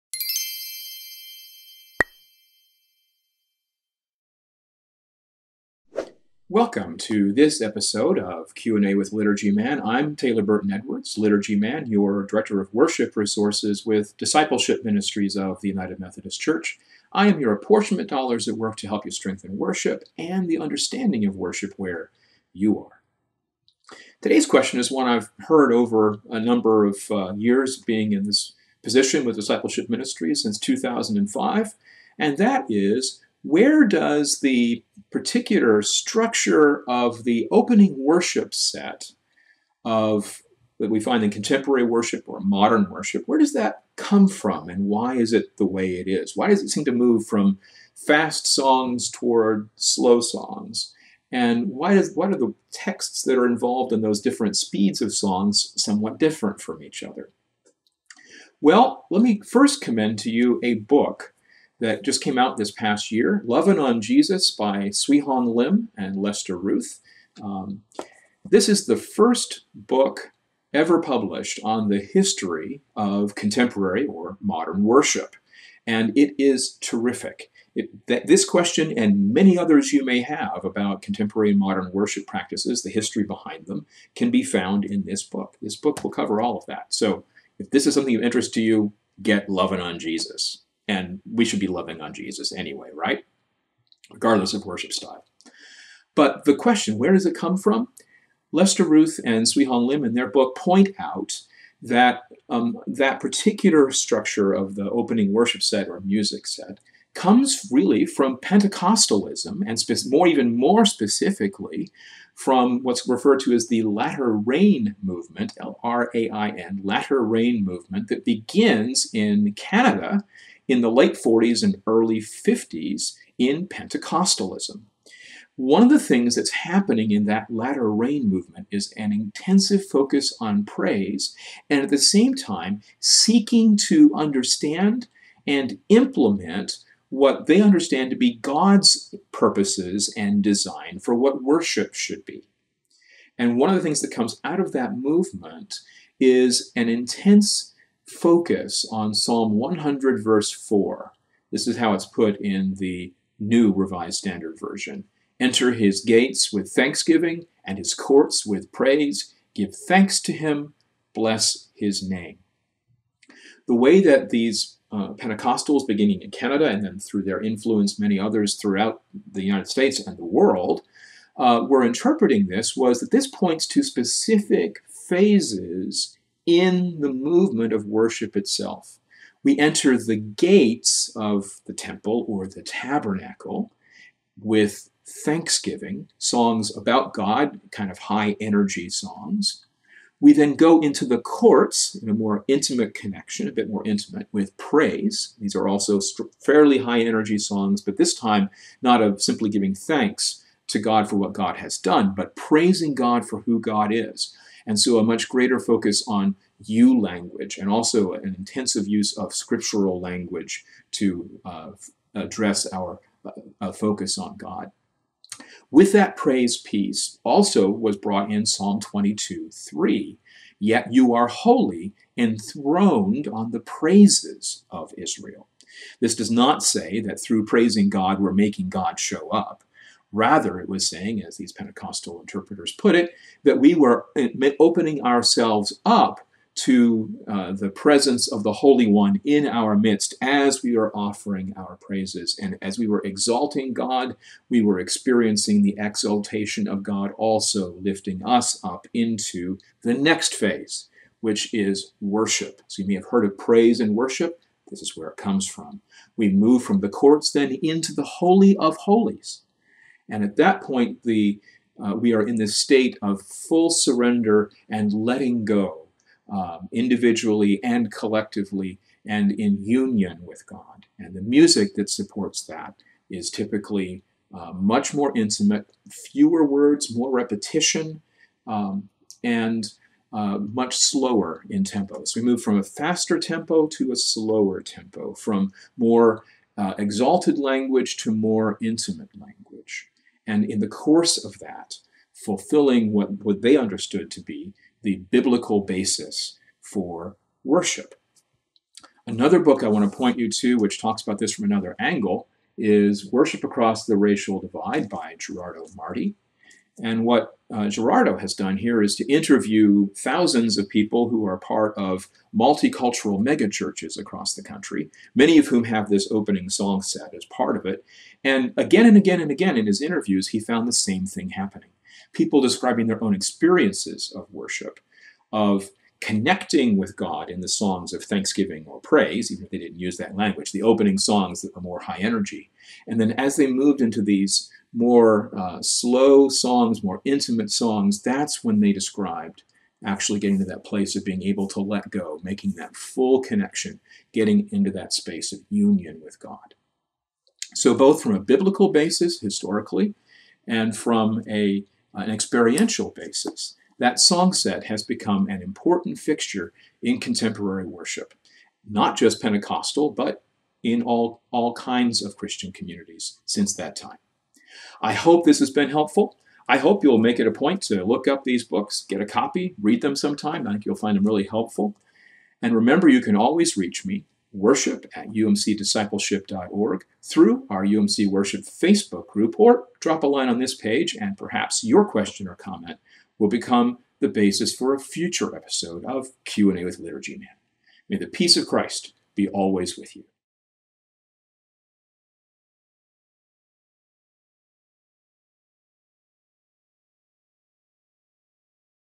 Liturgy Man: Where did the opening music set in contemporary worship come from?
Q&A with Liturgy Man is a series of 3-5 minute videos in which Taylor Burton-Edwards answers questions about United Methodist Worship that he receives in a variety of ways from United Methodists around the connection.